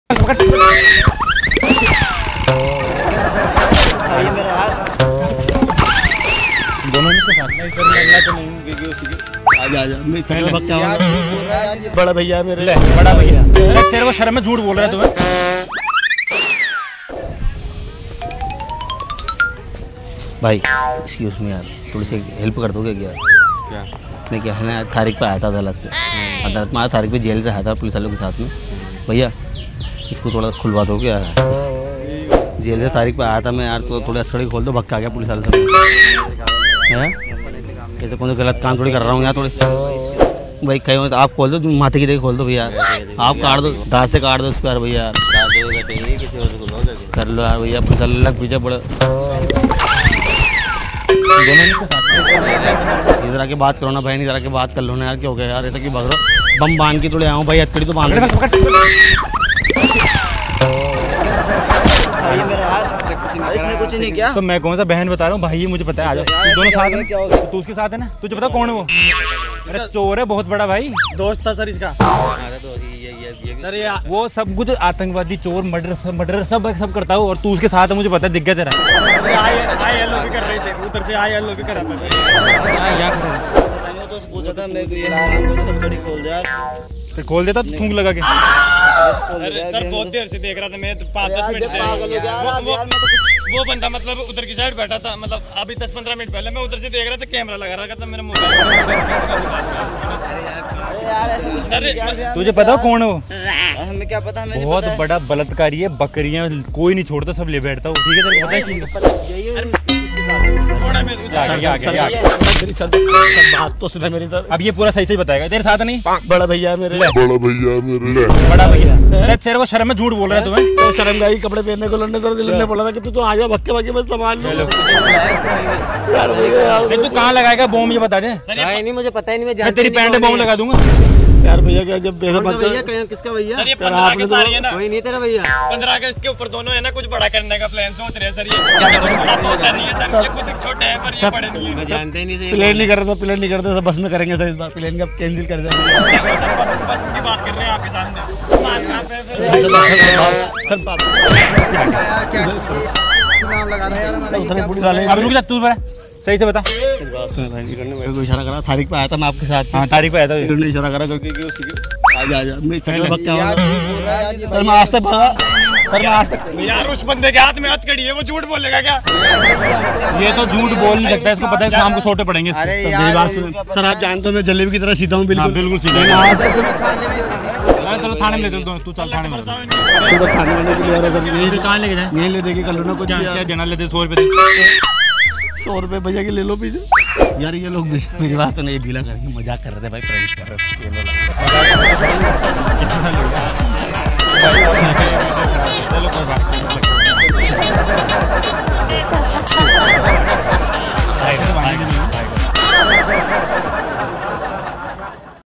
Fake Police Prank | Bhasad New Ft. @AYANPRANKTV | Pranks in India2024
We are back with another hilarious prank video."

Actor - Ali Pathan, Deepak Rajasthani & Ayan @AYANPRANKTV
Produced by - Mohammed Sabir
Director - Mohammed Sabir
Editing - Mohammed Sabir
Camera - Sahil Khan & Vinay